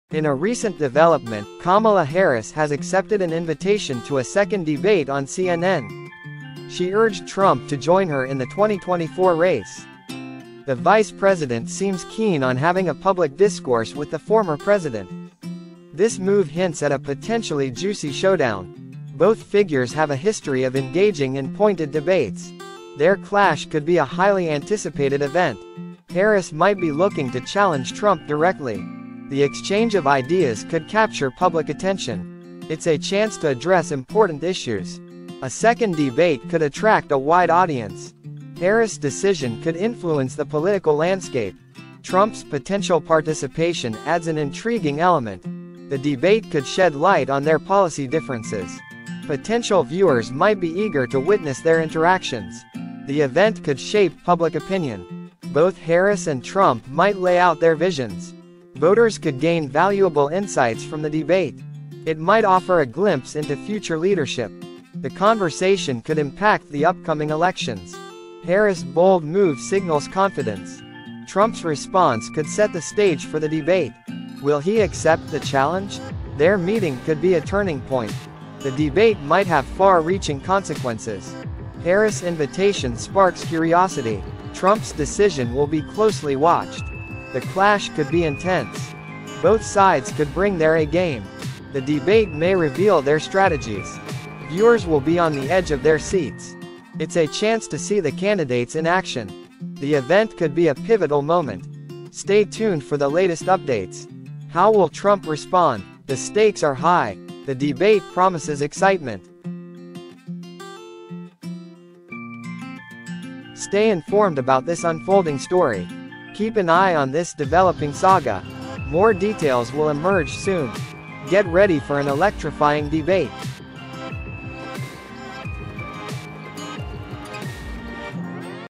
Harris Challenges Trump: 2024 Debate Showdown Awaits
Kamala Harris has accepted CNN's second debate invite and called on Trump to join—could this clash be the defining moment of the 2024 election?